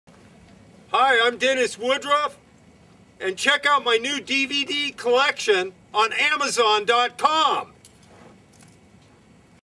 Troma releases the Dennis Woodruff DVD box set @ Amazon
Dennis Woodruff hypes up the new release of his new DVD boxed set collection released available at Amazon.com.